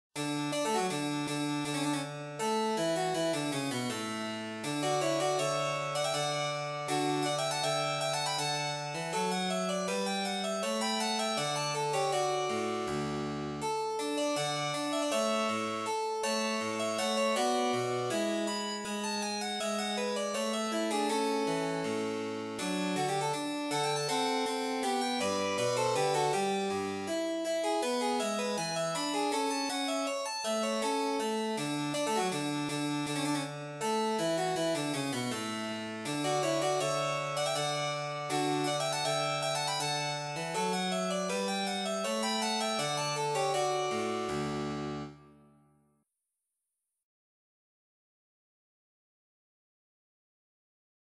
Kirnberger Minuet for Harpsichord
created by the musical dice game